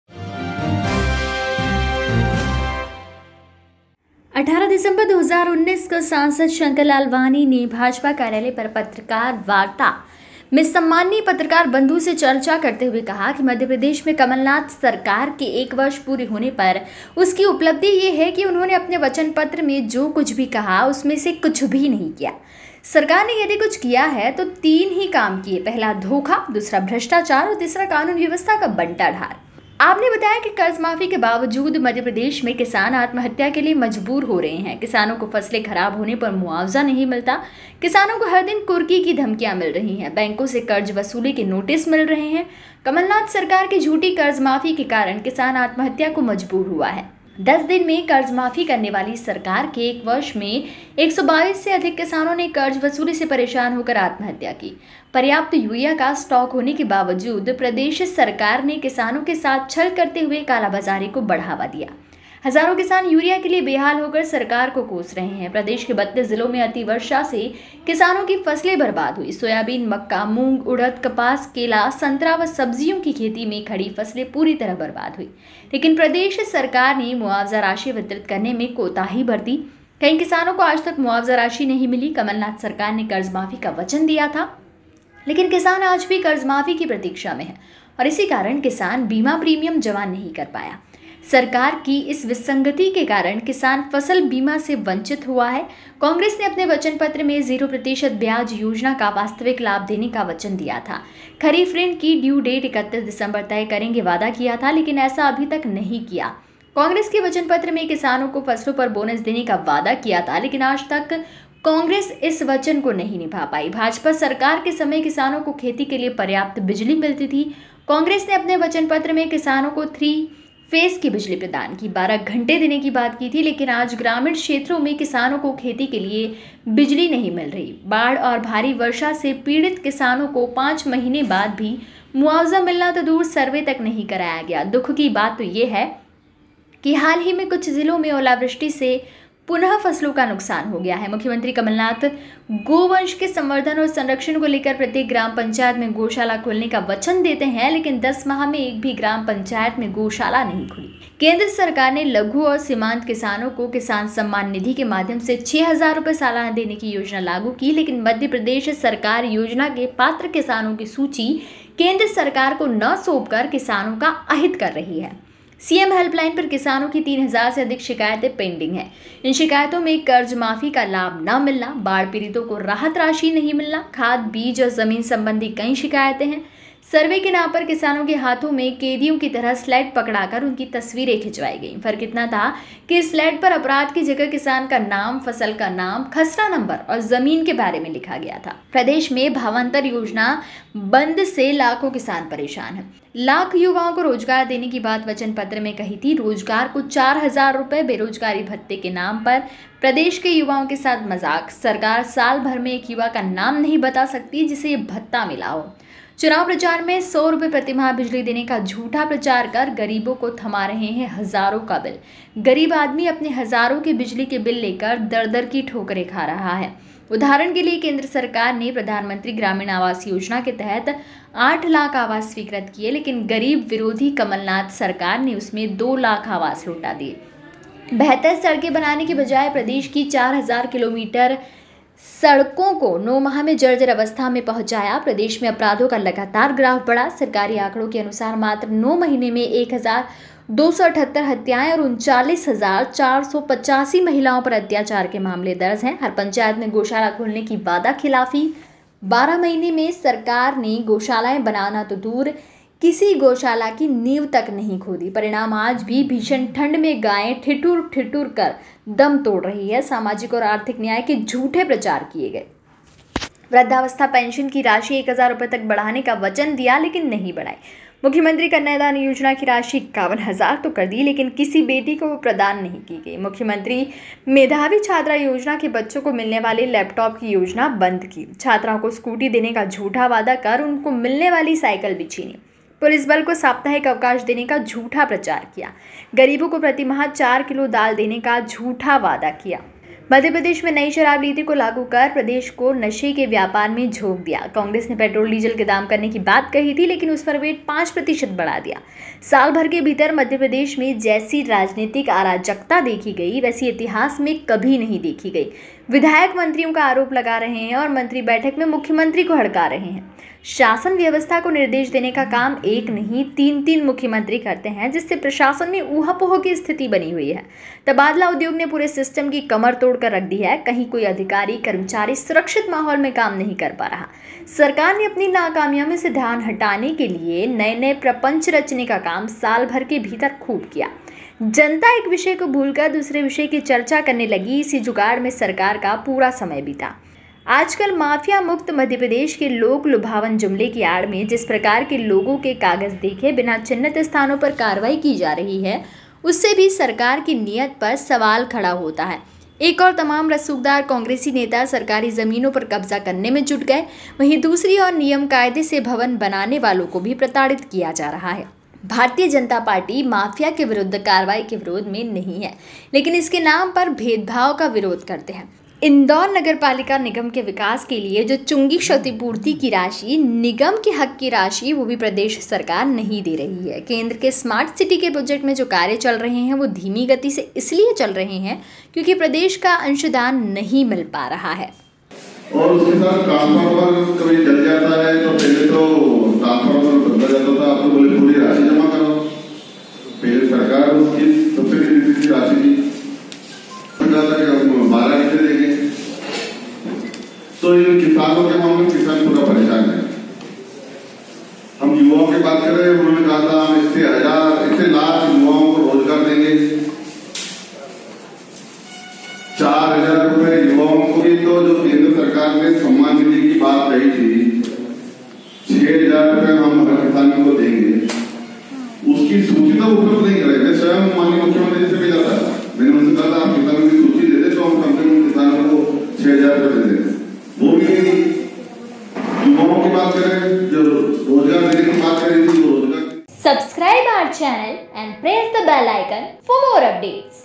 शंकर लालवानी ने कहा कमलनाथ सरकार नाकामयाब
पत्रकार वार्ता में उन्होंने लगाए आरोप